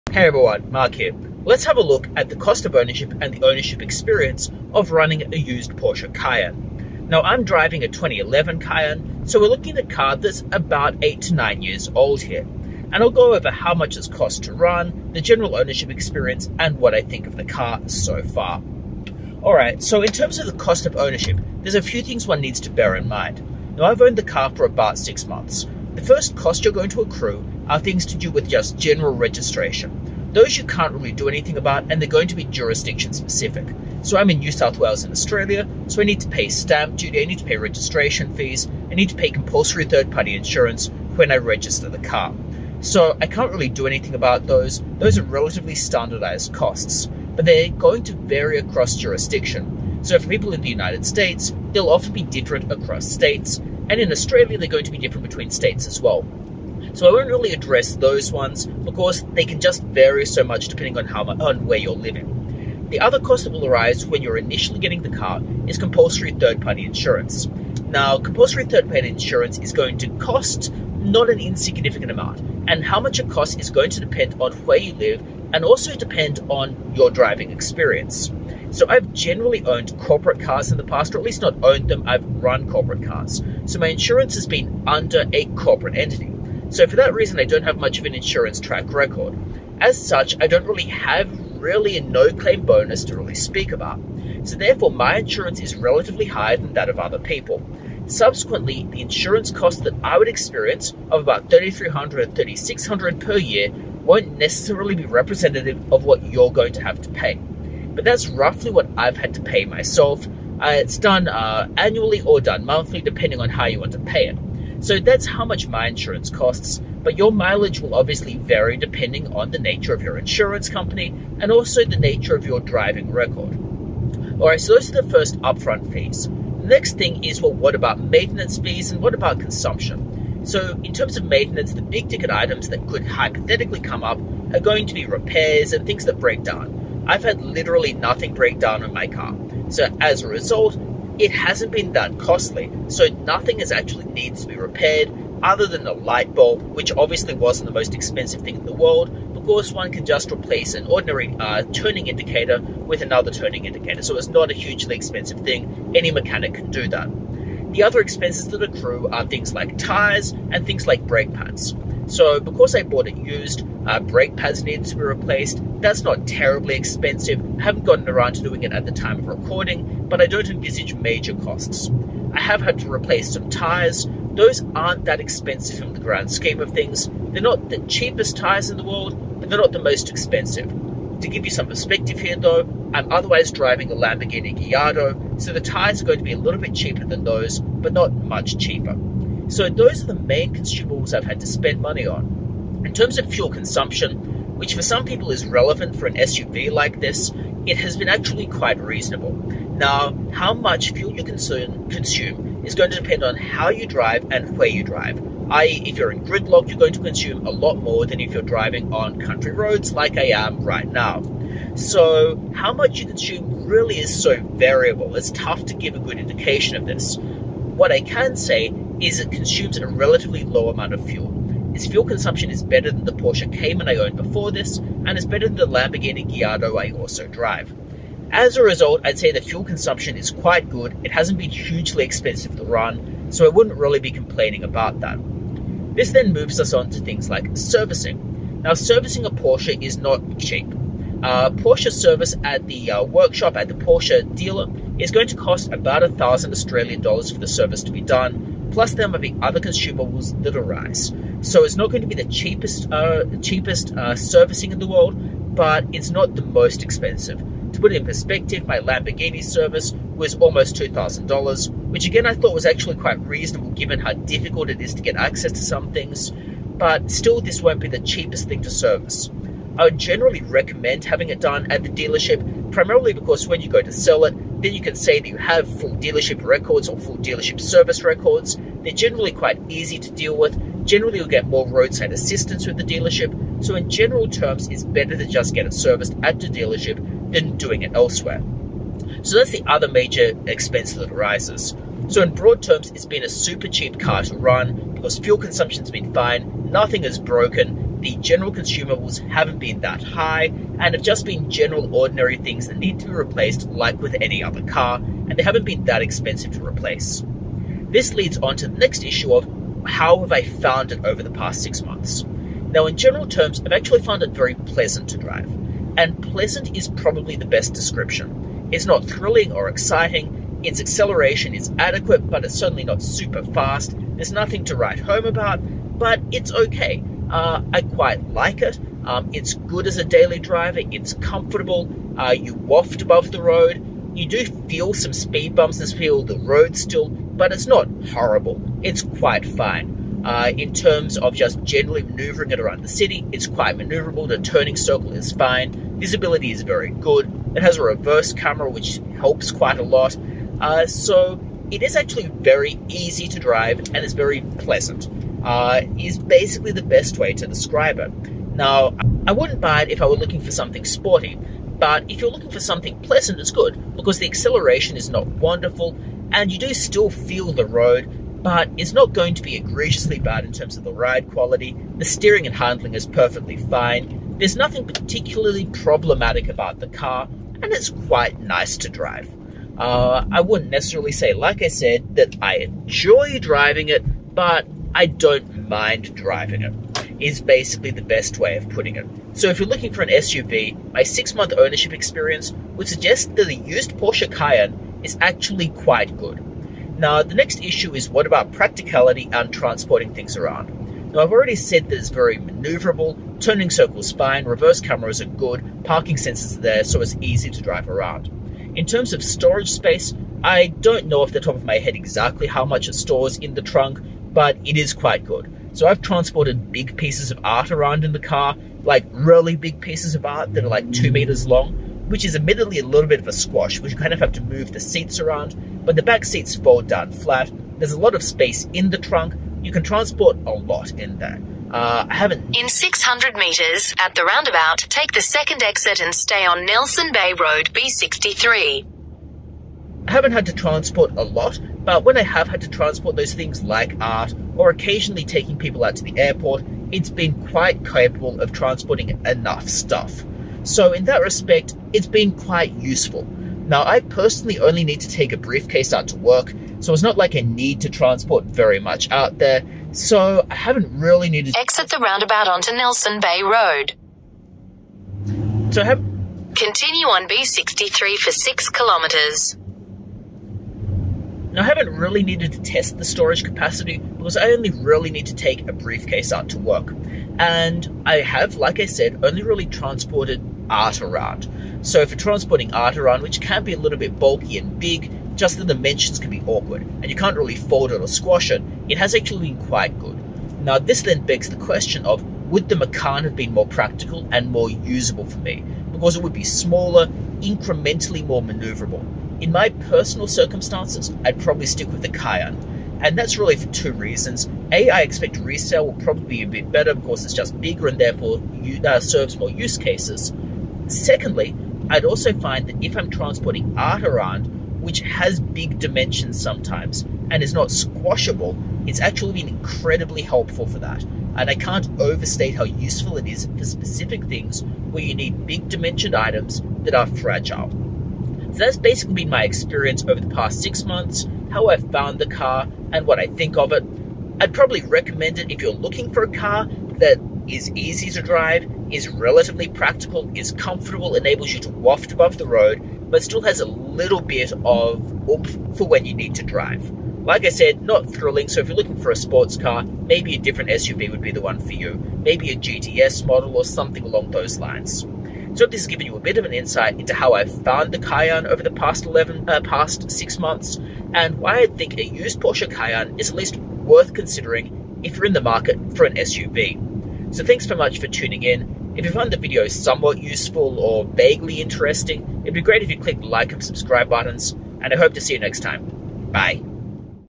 Porsche Cayenne: 6 month ownership experience and cost
What is it like to own and drive a Porsche Cayenne for Six Months? What is the cost of ownership for a used Porsche Cayenne?

This video discusses my experience with a used Porsche Cayenne 92A (from 2011). In general terms, the car has been very pleasant to use and quite cost effective to own.